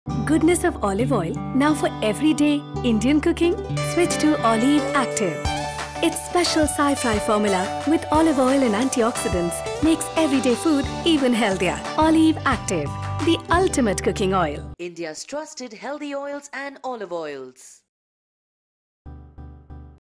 Oleev Range 20sec- English
Oleev Active is a Multisource cooking oil with a unique blend of Olive Oil and Rice Bran Oil, specially created to fulfill the demands of a modern and healthy lifestyle. It has an Energocules Formula that will ensure that you stay active and energized throughout the day and celebrate every moment of your life with nutrition that is real. It is an ideal edible oil for everyday cooking.